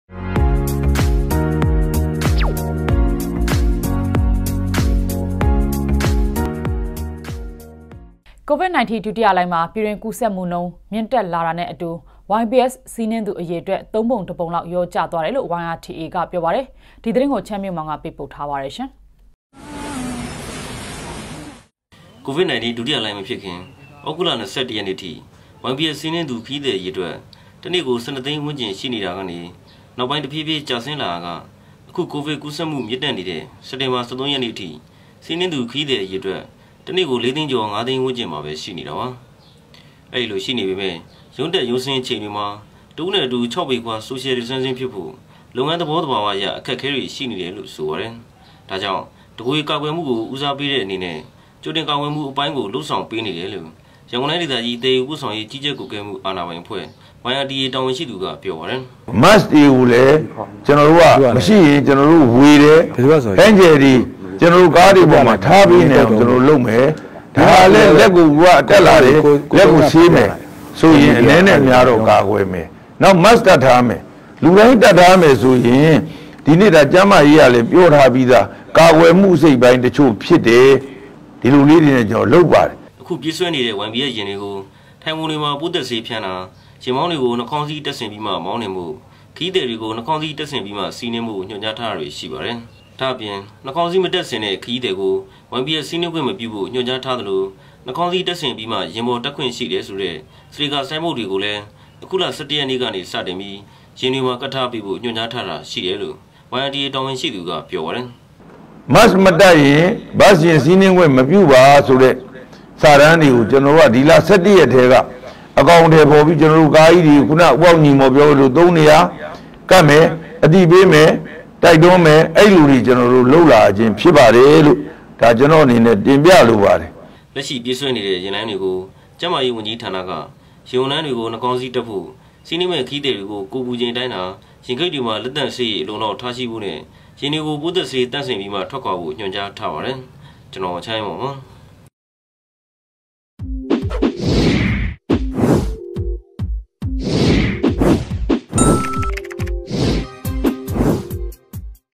DVB - ကိုဗစ်ဒုတိယလှိုင်းမှာ YBS စီးနင်းသူ သုံးပုံတစ်ပုံလောက် လျော့ကျ
ကိုဗစ် ၁၉ ဒုတိယလှိုင်းမှာ ပြည်တွင်းကူးစက်မှုနှုန်းမြင့်တက်လာတာနဲ့အတူ YBS စီးနင်းသူအရေအတွက် သုံးပုံတစ်ပုံလောက် လျော့ကျသွားတယ်လို့ YRTA ကပြောပါတယ်။ ဒီသတင်းကိုချမ်းမြေ့မောင်ပေးပို့ထားတာပါ။